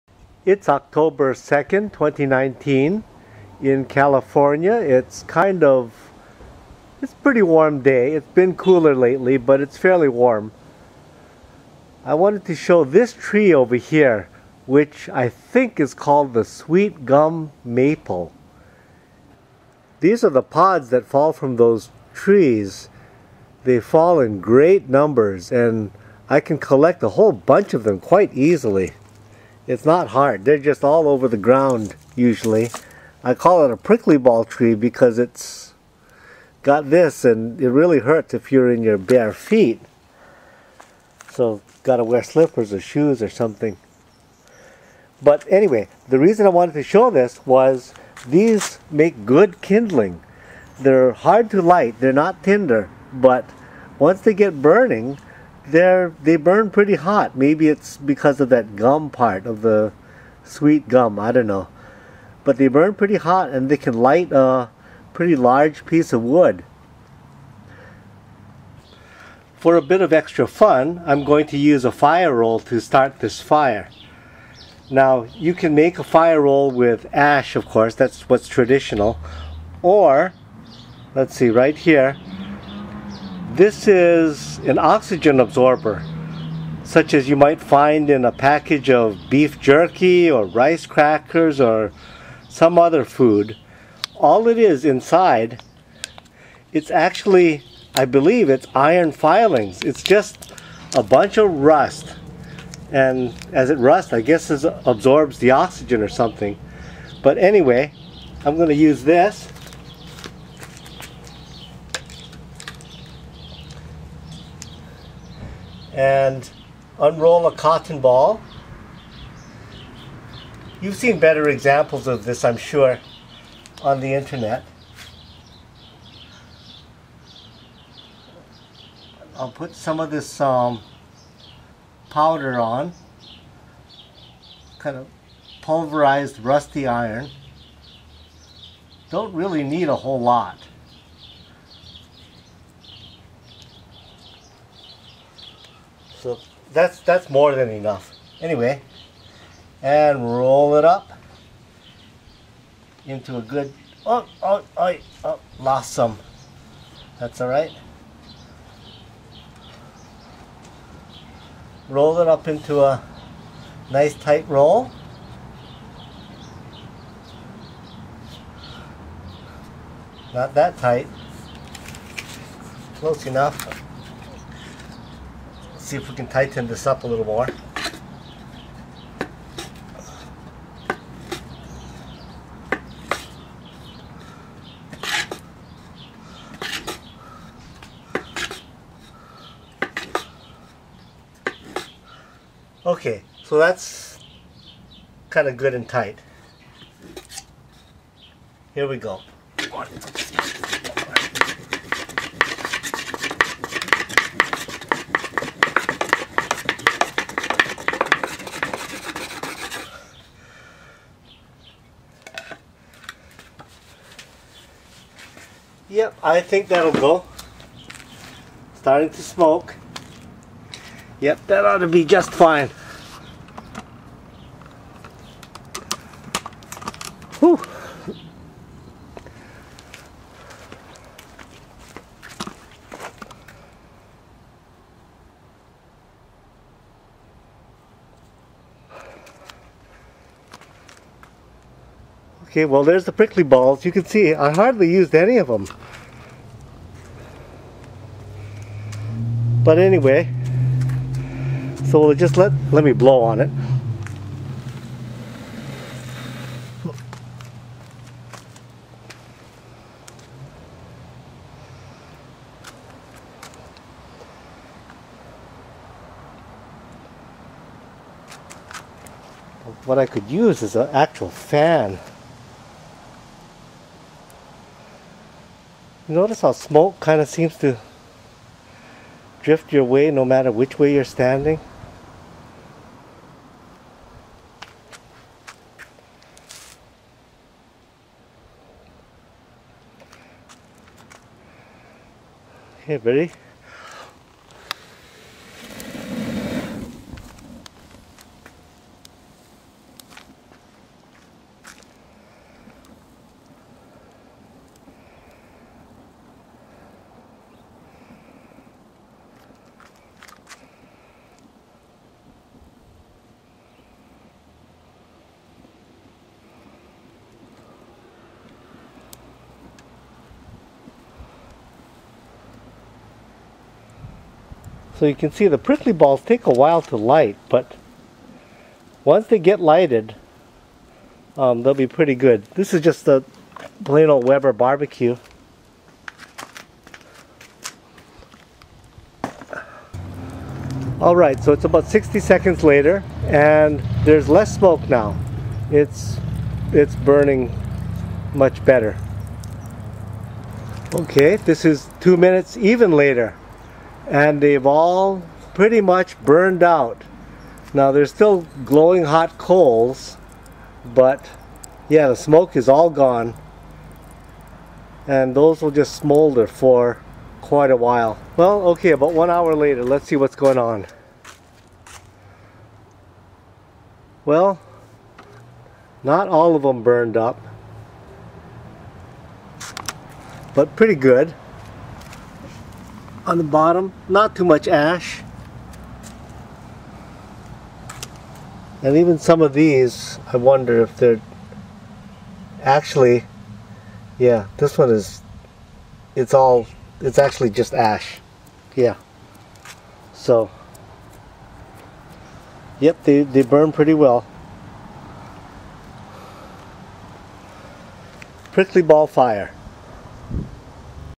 Prickly Balls as Kindling with Fire Roll
The seed pods of the Sweet Gum Maple make good kindling, burning fast and hot, once they get started.  Just for fun, I started them with a fire roll, using pine needles for tinder.